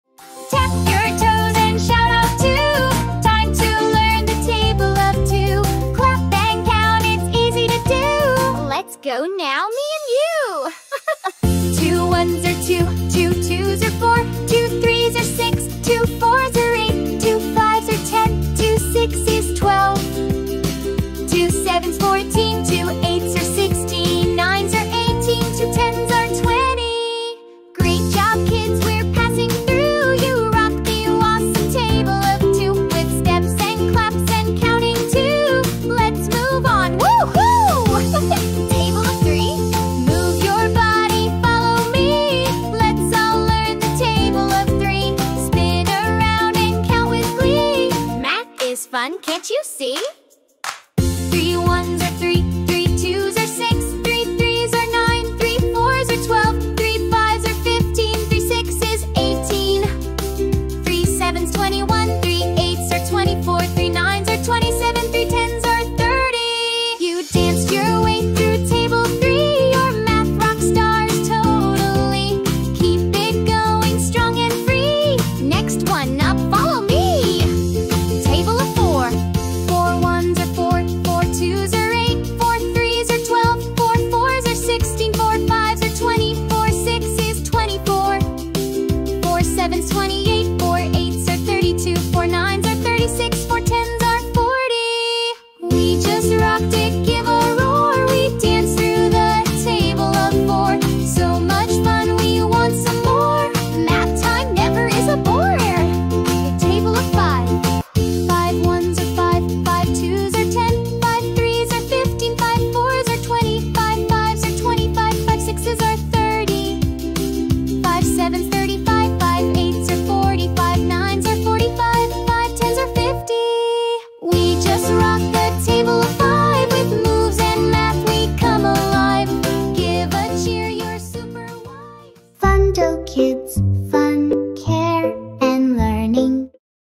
Table of 2 to 5 | Learn Table of 2 to 5 with fund song | Recite table of 2 to 5
Let’s make learning fun for little minds! 🎉
In this colorful and energetic video, kids will learn the tables of 2, 3, 4, and 5 through catchy music, dancing numbers, and cheerful visuals. Perfect for kids aged 4 years and above. Help your child build a strong math foundation while having loads of fun! 🧠🎵

👉 Ideal for kindergarten and early primary students.
👉 Watch, sing along, and repeat for easy memorization!